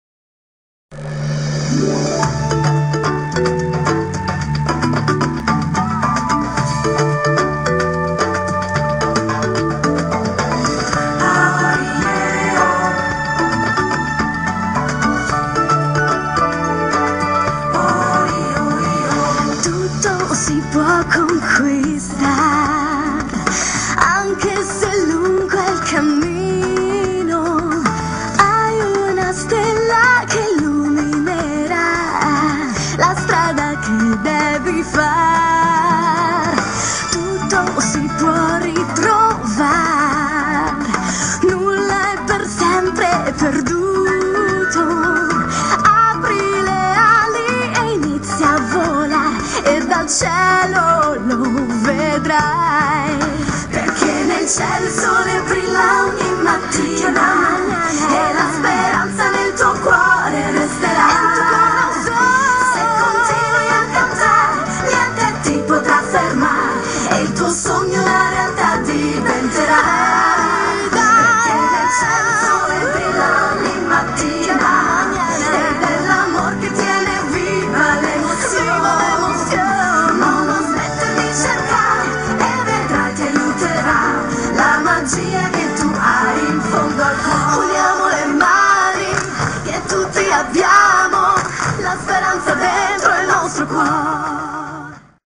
Concorso Del Mondo Di Patty _ By IlMondoDiPattyLL
partecipate in tanti,,lasciatemi un video di risposta e vi farò sapere ki saranno i 5 semifinalisti